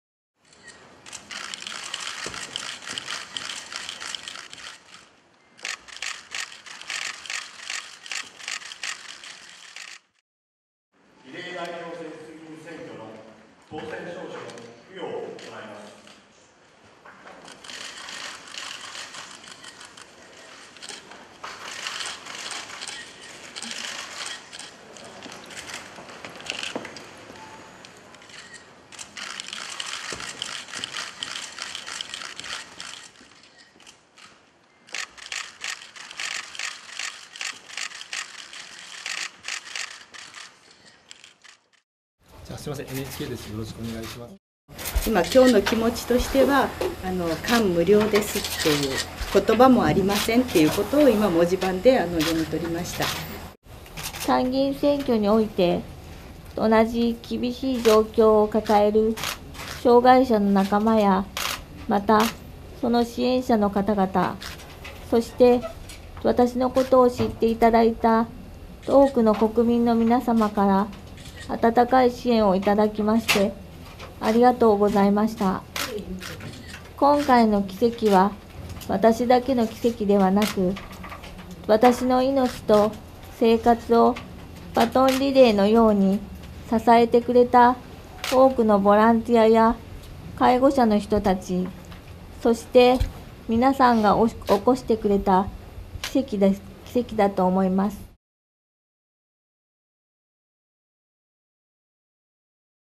参院比例当選者に証書 れいわの重度障害者ら
中央選挙管理会(宮里猛委員長)は26日、第25回参院選比例代表の当選者50人を告示し、総務省で、れいわ新選組から初当選した重い身体障害がある2人ら当選者本人、代理人に当選証書を渡した。